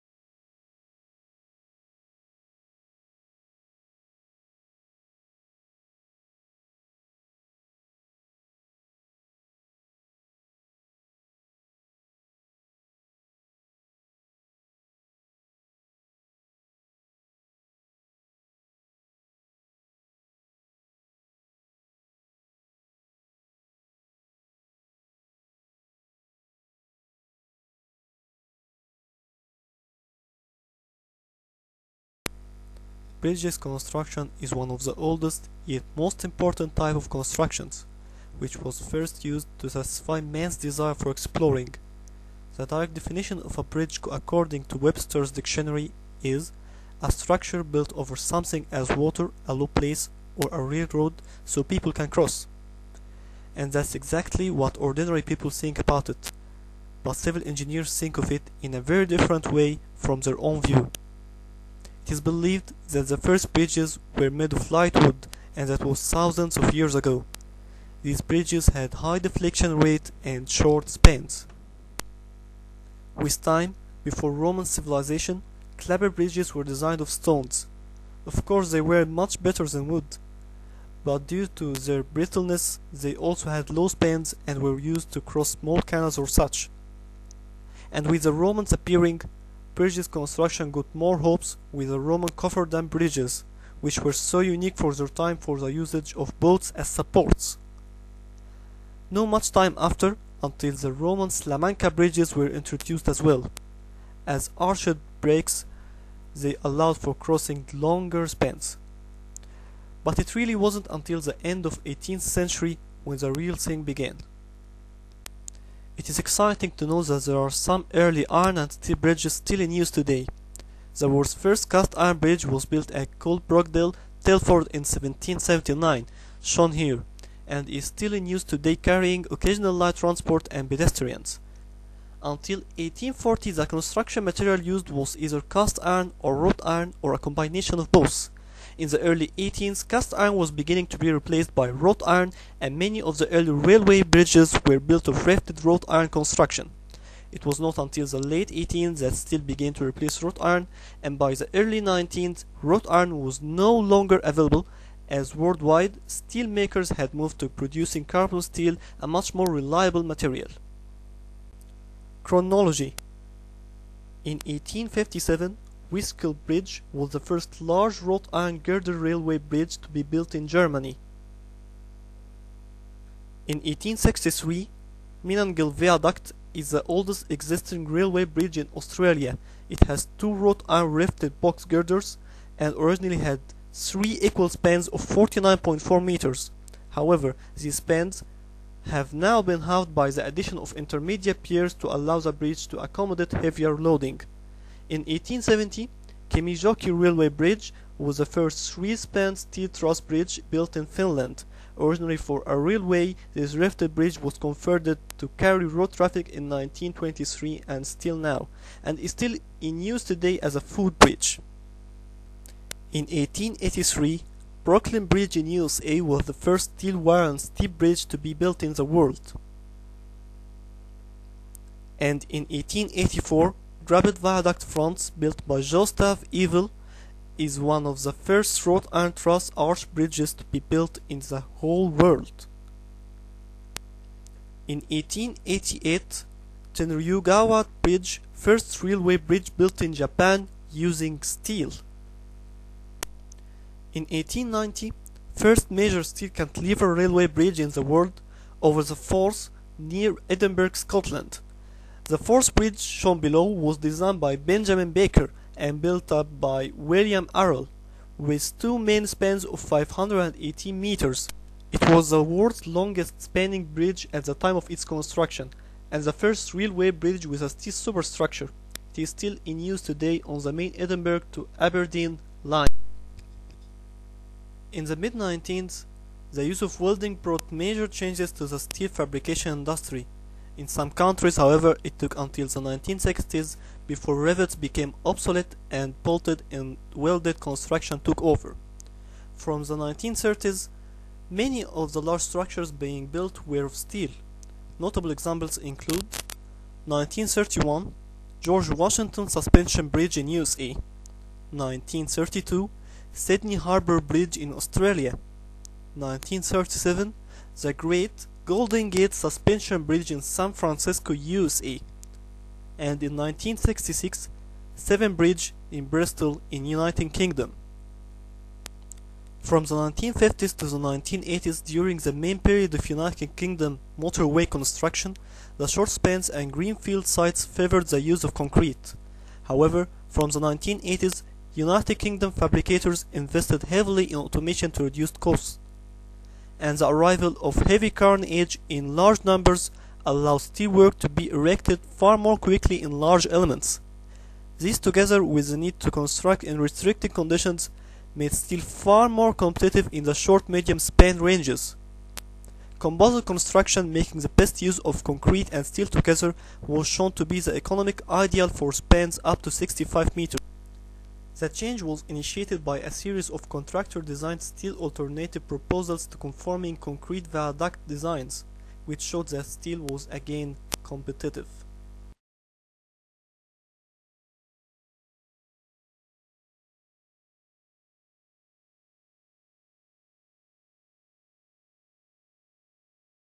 Steel Bridges history
I was asked to do a power point presentation for my steel III project. But I also wanted to make a small video about bridges construction history by myself. Thank's to having very little time left I rushed that video that it looks like crap :p . But I uploaded it here anyway in case it may serve anyone in any way. Notice that many of the ideas were removed (Specially most sprites scenes were removed). Speciall thank's to my great professor Maguid who used to teach me at HTI